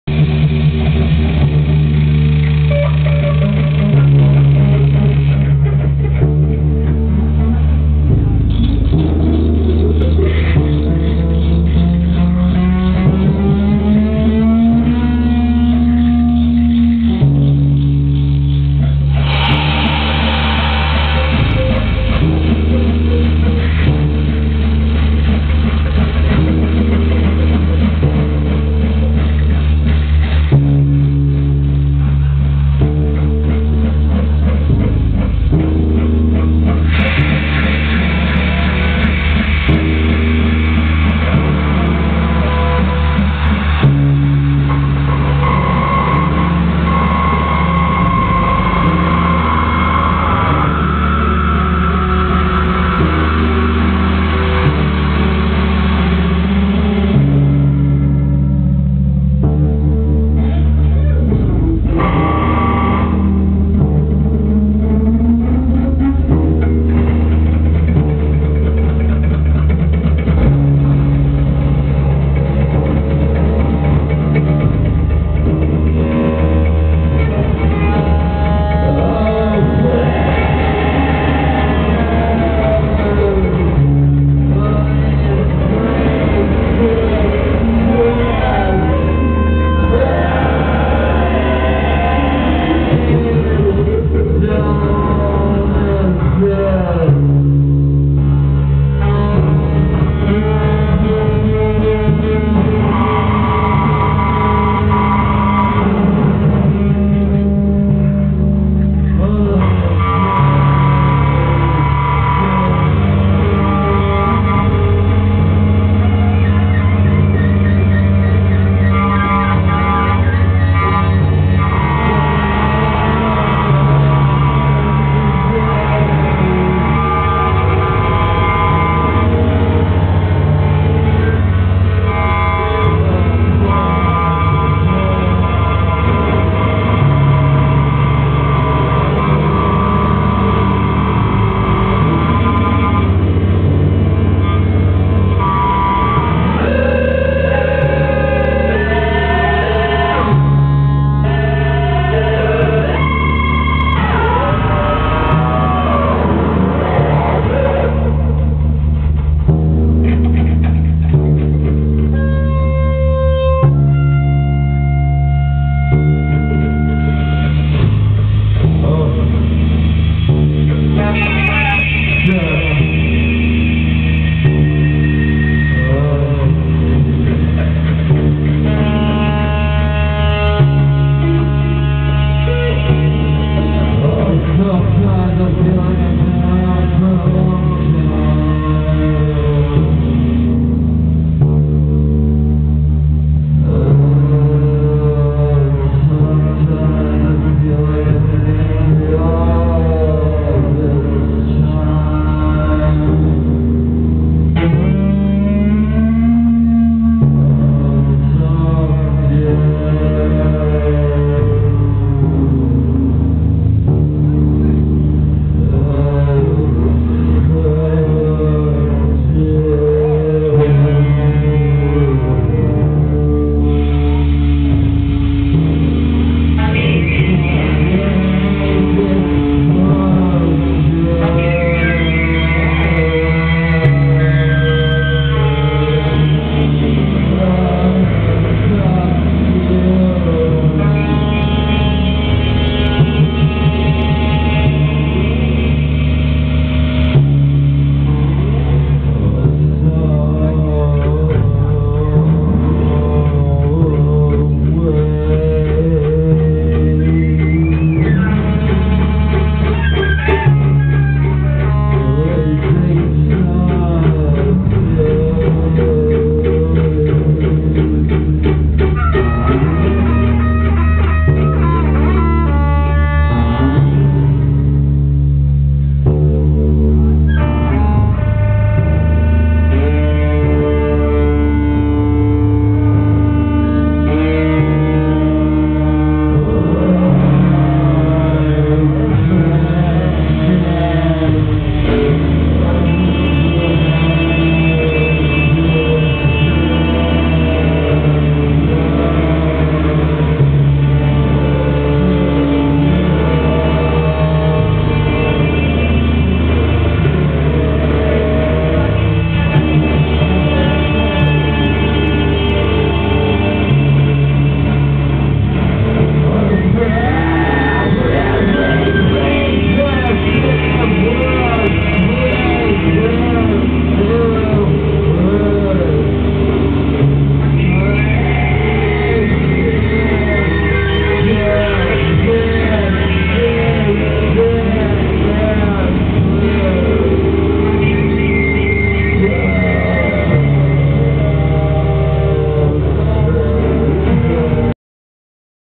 Wolf Eyes at CAID 10.8.10
The set opener from Wolf Eyes live at the Hermitage Tapes Benefit at C.A.I.D. in Detroit, Michigan on October 8th, 2010.